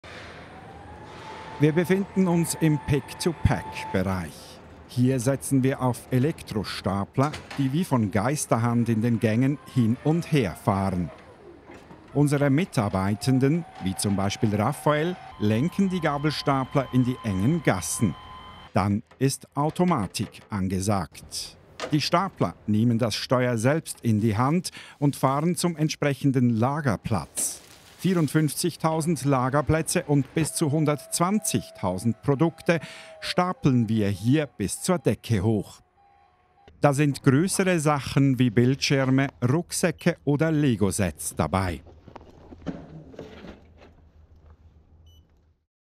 Einblick ins Warenlager: Mit dem Gabelstapler in die Höhe
Hier, im Pick to Pack-Bereich, werden grössere Produkte mit Gabelstaplern eingelagert und bei Bedarf wieder herausbefördert. 54’000 Lagerplätze gibt es hier und zu Spitzenzeiten fahren XX Gabelstapler gleichzeitig durch die Gänge.